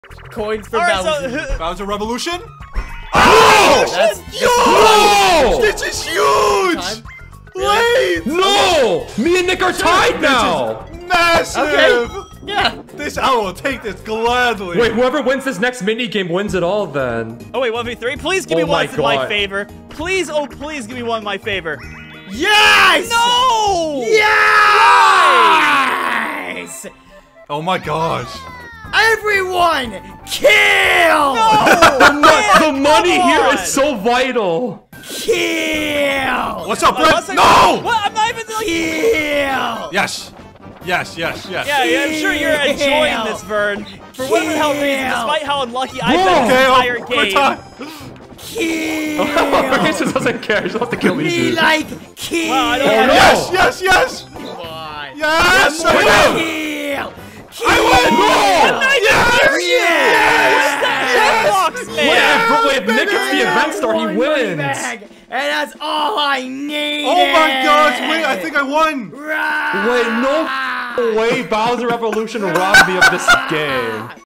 Party Crashers Highlights - Bloodthirsty Vernias
Looks like all those cursed dice blocks that Sophist gave him have pushed him over the edge.